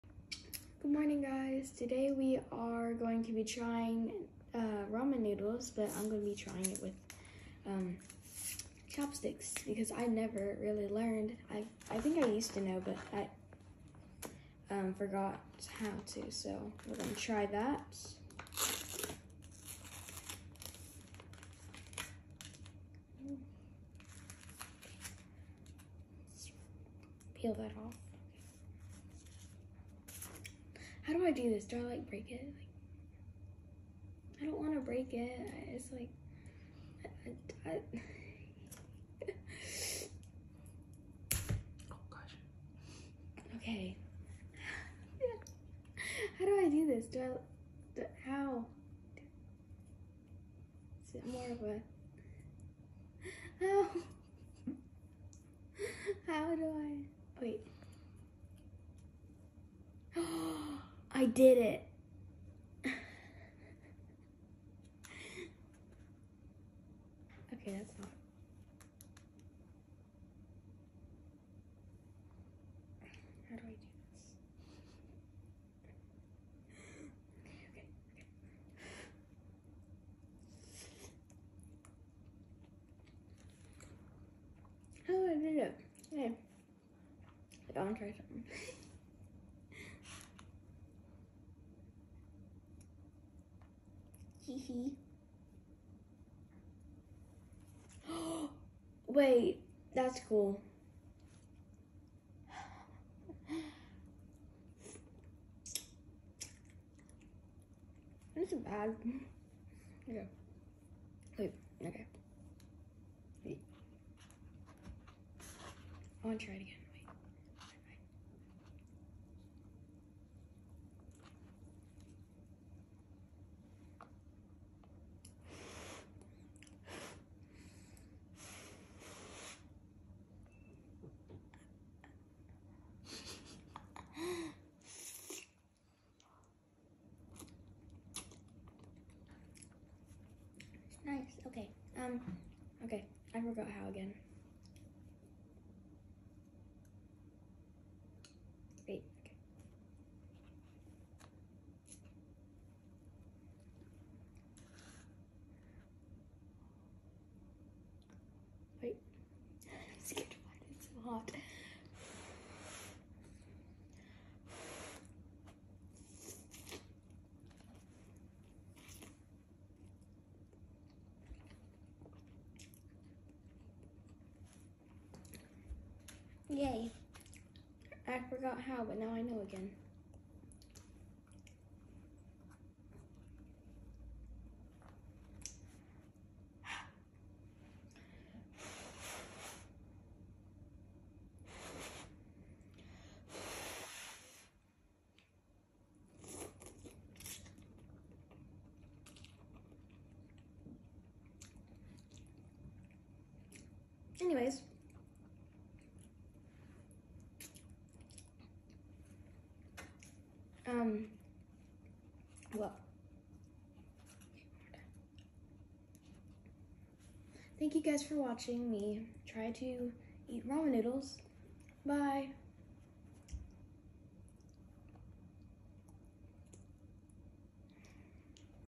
Trying chopsticks with ramen noodles
Nadia wanted to try to eat her noodles with chopsticks.